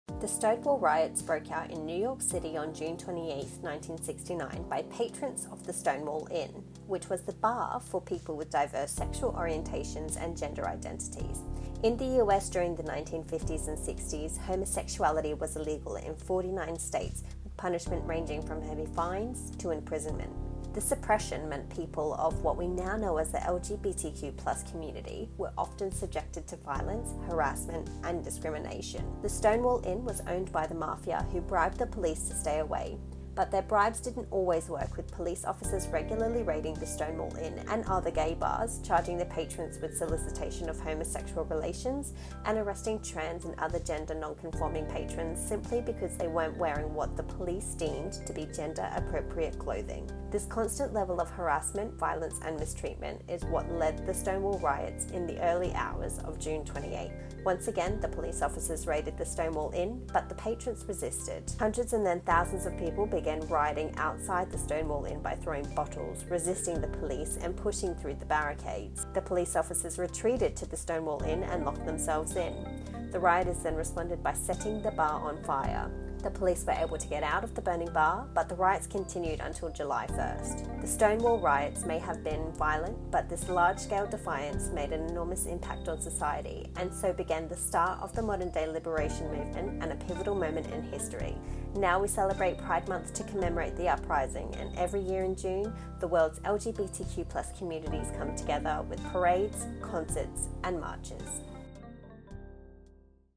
The Stonewall riots - what happened and why | VideoScribe
The Stonewall riots is the reason why Pride month takes place in June and to understand how these protests changed LGBTQ+ rights for a lot of people in the US and beyond we've created a short explainer video.